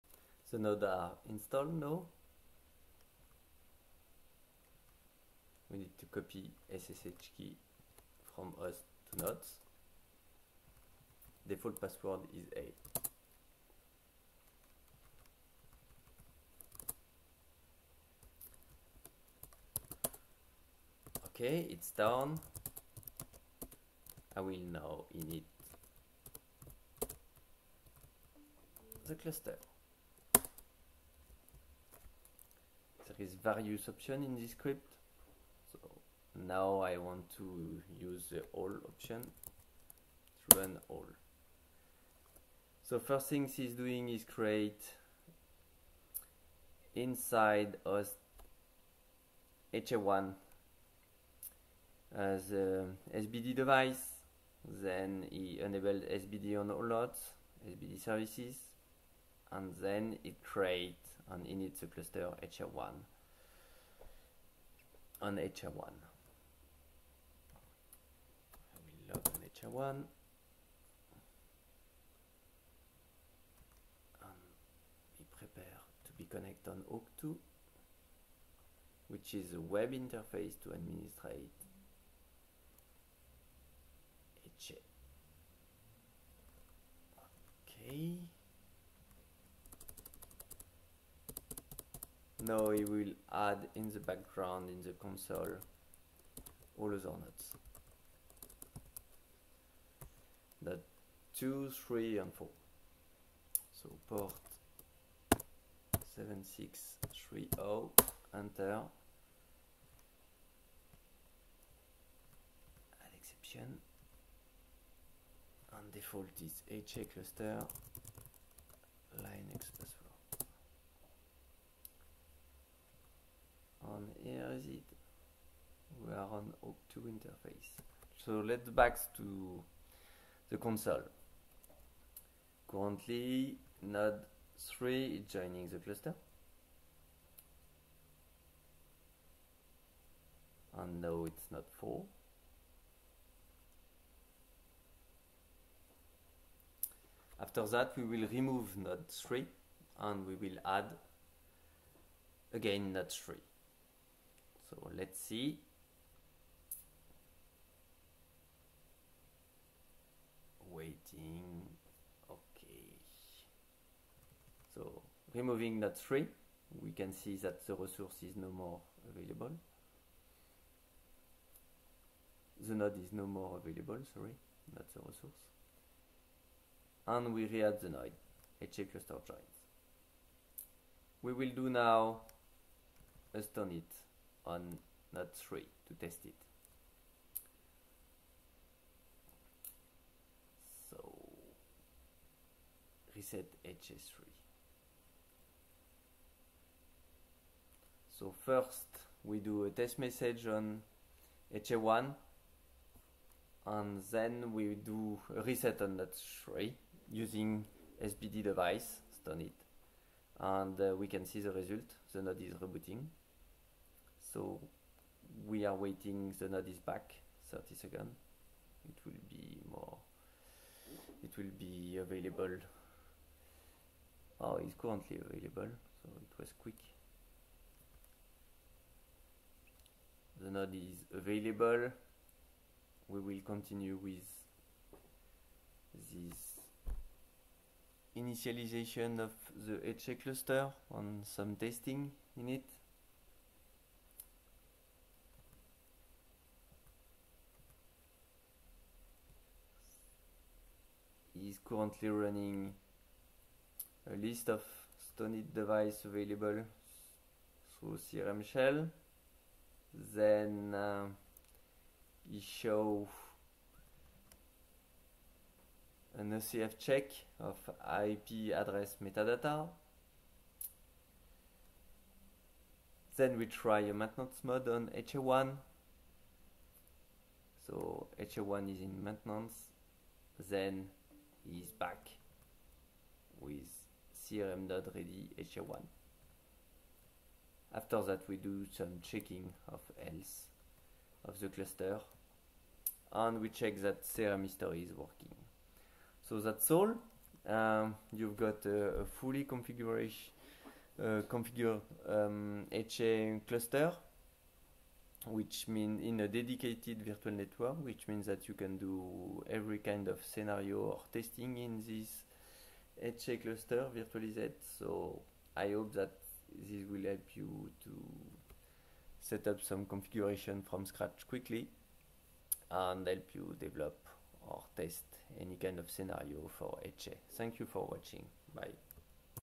Deploy an HA SUSE Linux Enterprise Cluster in Virtual Machines (2/3)
* Deployment of HA Virtual Machine

This Demo is done using SUSE Linux Entreprise Server 12 SP2 BETA3 + HA product. You can register the SUSE Linux Beta program